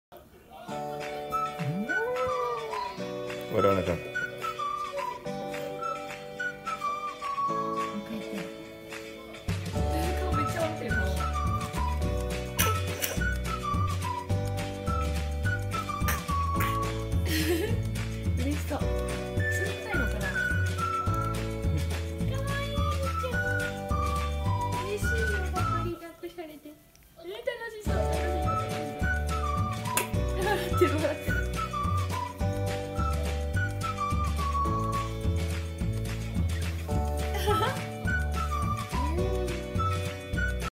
パパにお腹をくすぐられて笑うゆいちゃん20191103
ゆいちゃんInstagram
「おかっぱミユキ＆まんじゅう」